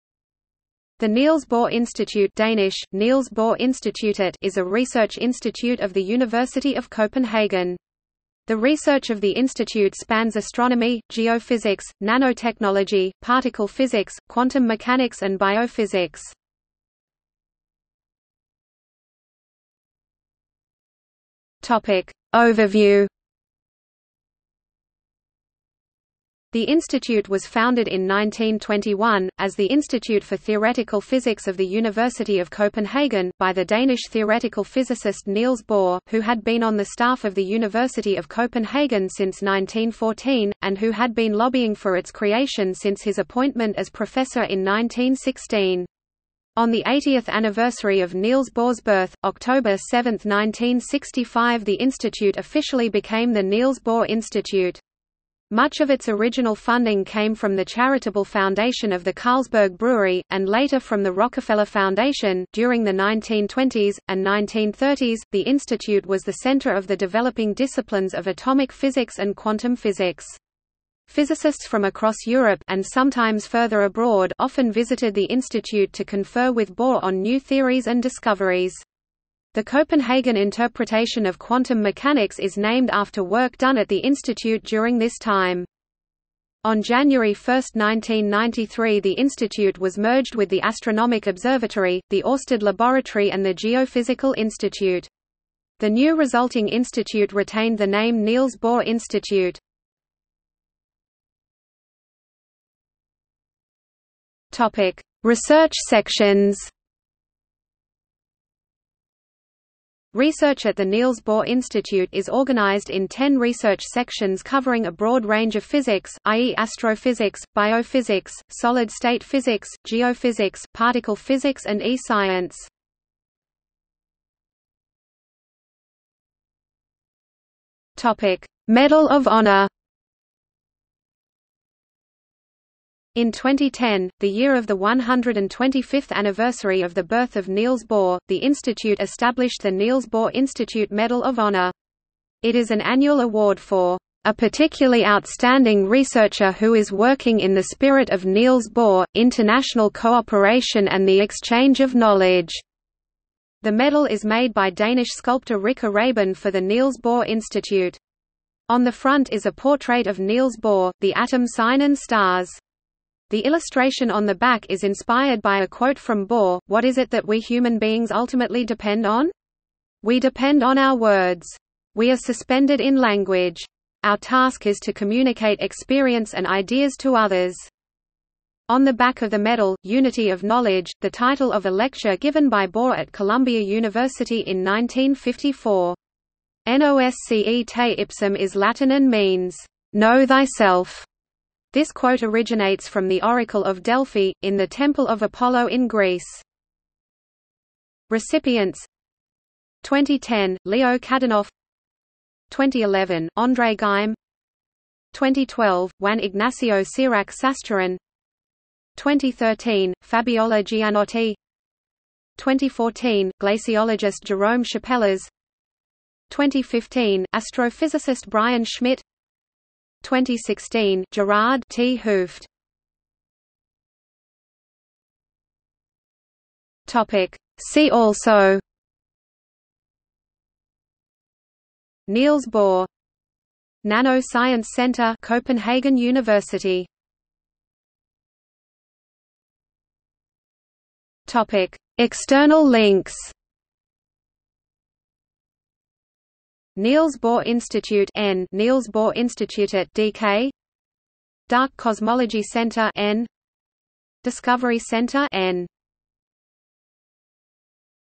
Niels Bohr Institute for Astronomy, Geophysics and Physics | Wikipedia audio article
00:00:18 1 Overview
00:01:41 2 Research sections
00:02:04 3 Medal of Honour
00:03:43 4 See also
00:03:57 5 External links

- Socrates

SUMMARY
The Niels Bohr Institute (Danish: Niels Bohr Institutet) is a research institute of the University of Copenhagen. The research of the institute spans astronomy, geophysics, nanotechnology, particle physics, quantum mechanics and biophysics.